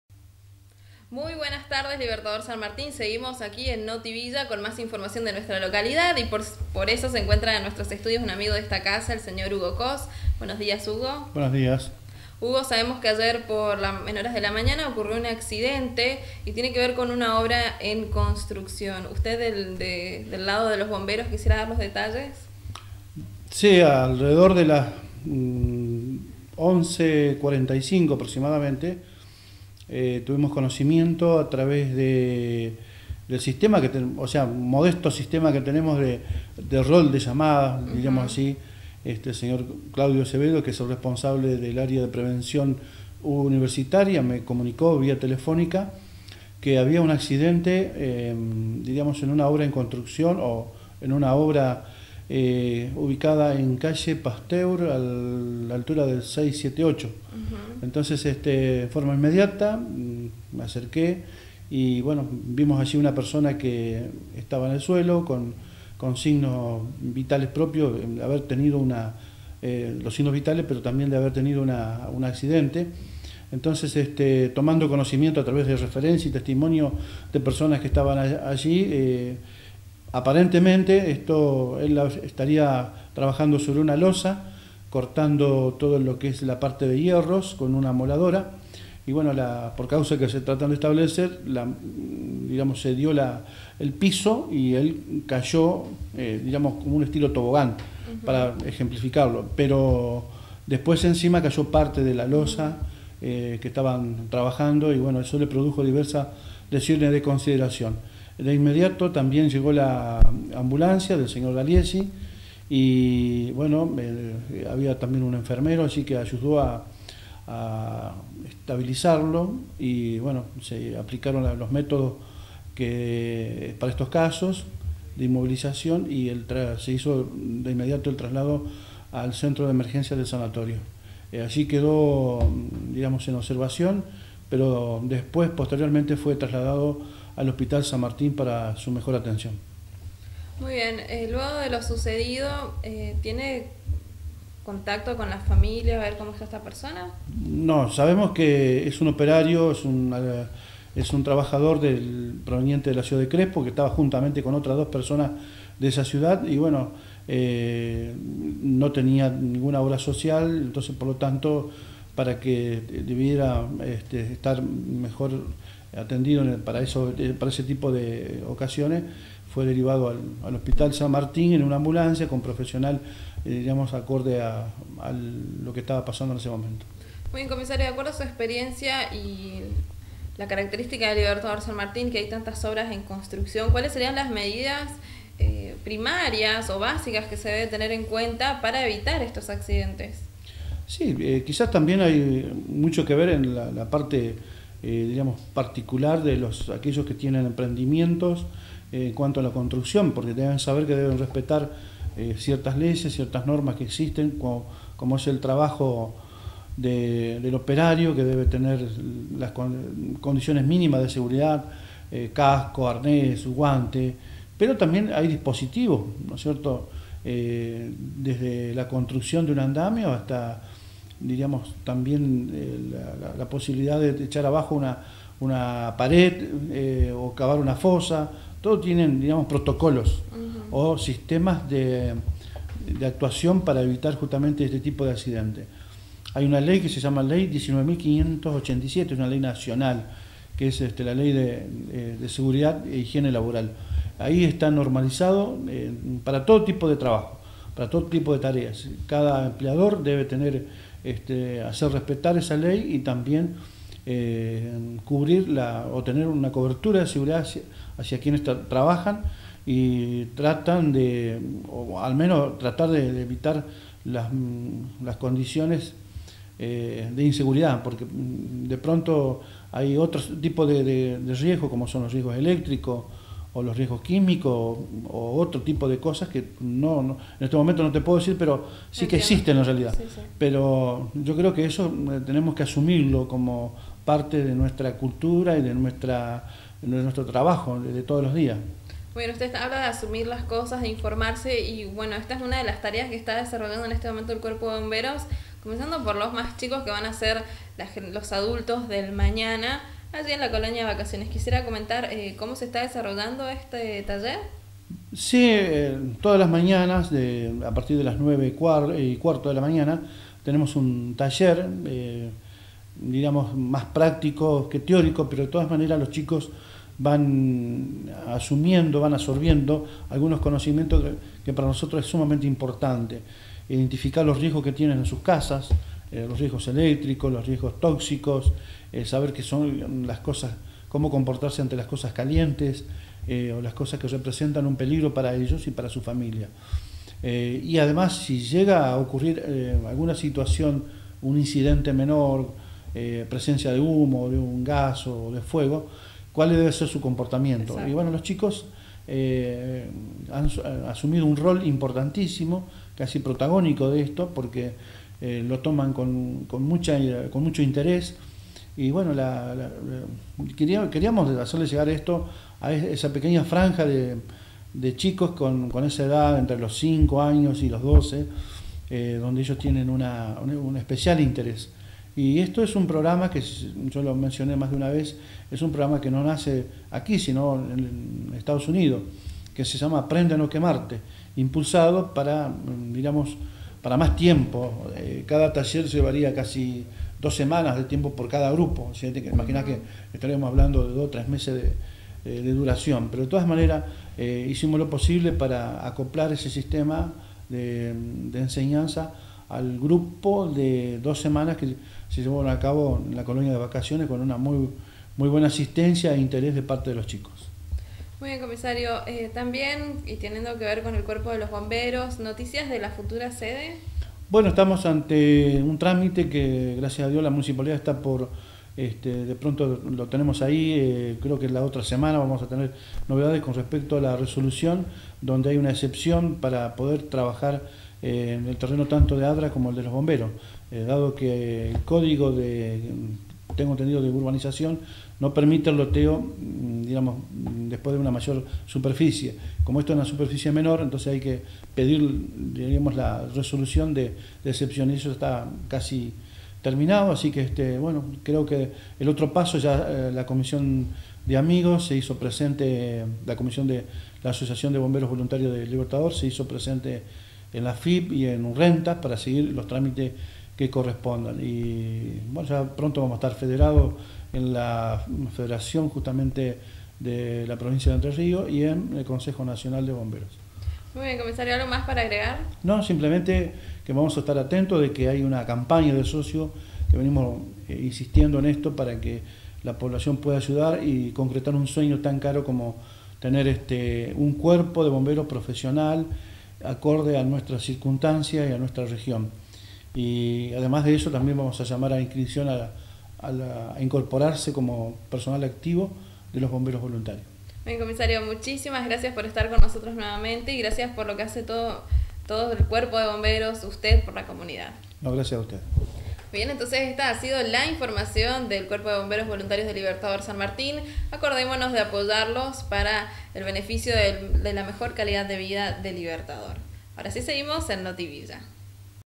Bomberos Libertador San Martín
El Crio. Mayor Hugo Kos hace referencia a un accidente protagonizado por un empleado de la construcción, en el momento en el que se disponía a demoler parte de una vivienda particular. Además de otras novedades de los bomberos voluntarios.